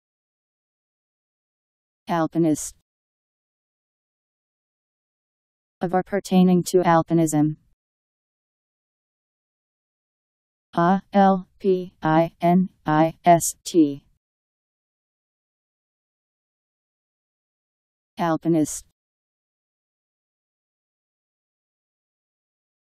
Alpinist Meaning
Video shows what alpinist means. Of or pertaining to alpinism..  Alpinist Meaning. How to pronounce, definition audio dictionary. How to say alpinist. Powered by MaryTTS, Wiktionary